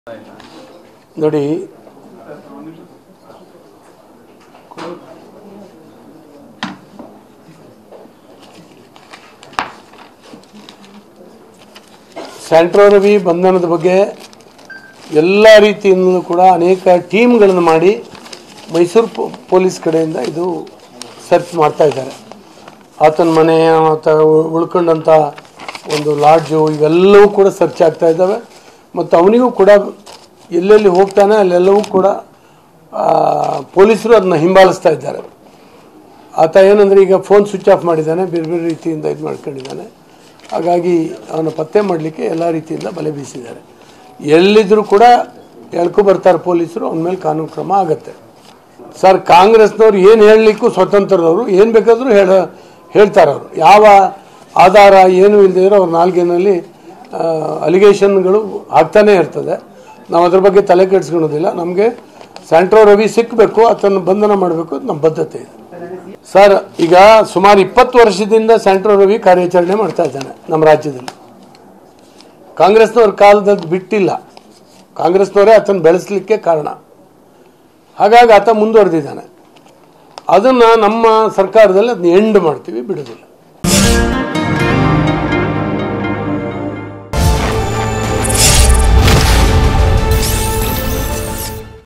Araga Jnanendra: ಸ್ಯಾಂಟ್ರೋ ರವಿ ಎಲ್ಲೇ ಇದ್ರೂ ಪೊಲೀಸರು ಎಳೆದುಕೊಂಡು ಬರ್ತಾರೆ
ಸ್ಯಾಂಟ್ರೋ ರವಿ ಬೆಳೆಯಲು ಕಾಂಗ್ರೆಸ್​ನವರೇ ಕಾರಣ ಎಂದು ಹೋಂ ಮಿನಿಸ್ಟರ್ ಆರಗ ಜ್ಞಾನೇಂದ್ರ ವಾಗ್ದಾಳಿ ನಡೆಸಿದ್ದಾರೆ.. ಆತ ಬೆಳೆಯಲು ಕಾಂಗ್ರೆಸ್ ಕಾರಣ, ನಾವು ಅಂತ್ಯ ಮಾಡುತ್ತೇವೆ ಎಂದು ವಿಕಾಸಸೌಧದಲ್ಲಿ ಗೃಹ ಇಲಾಖೆ ಸಚಿವ ಆರಗ ಜ್ಞಾನೇಂದ್ರ ಹೇಳಿಕೆ ನೀಡಿದ್ದಾರೆ. ಸ್ಯಾಂಟ್ರೋ ರವಿ ಬಂಧಿಸಲು ಅನೇಕ ಕಡೆ ಹುಟುಕಾಟ ನಡೆಸಿದ್ದಾರೆ. ಅನೇಕ ತಂಡ ಮಾಡಿ ಮೈಸೂರು ಪೊಲೀಸರು ಹುಡುಕಾಟ ನಡೆಸಿದ್ದಾರೆ. ಸ್ಯಾಂಟ್ರೋ ರವಿ ಎಲ್ಲೇ ಇದ್ರೂ ಪೊಲೀಸರು ಎಳೆದುಕೊಂಡು ಬರ್ತಾರೆ
ಎಂದ್ರು.

Credits: Regional|Manoharsidlaghatta/Producer|jayaram /Videoeditor